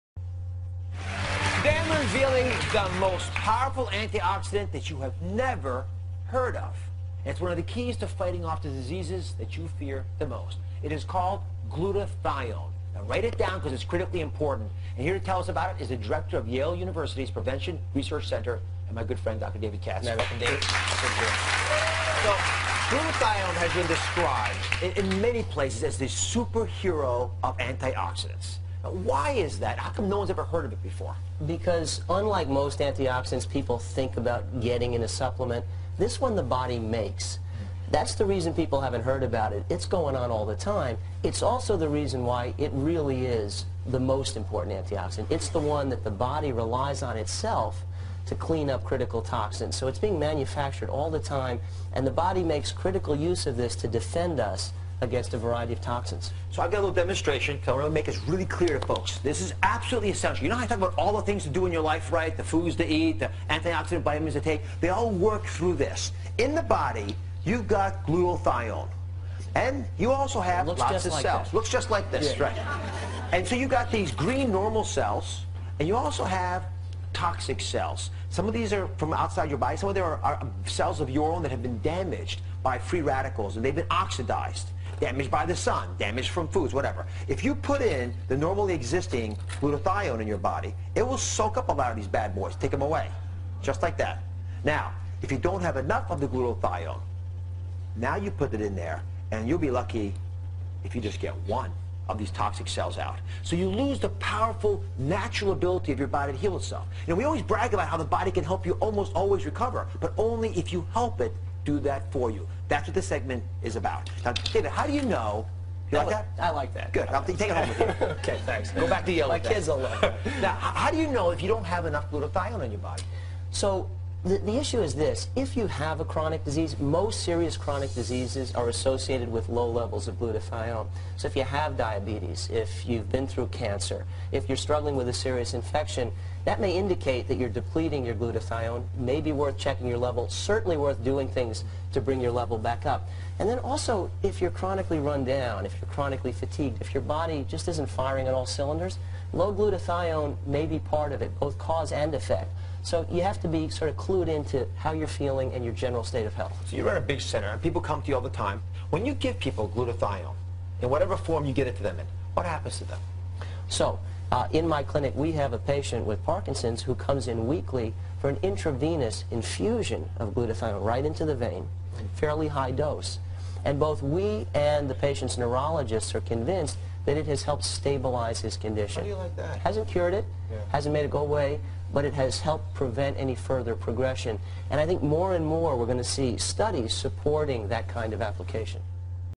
Dr. Oz Explains Glutathione
Dr. Oz and Director of Yale University's Prevention Research Center Dr. David Katz explain the powerful antioxidant glutathione and its health applications.

Infuze MD in Milpitas, CA offers glutathione IV supplements for patients with cancer, parkinson’s disease, heart disease to boost the immune system.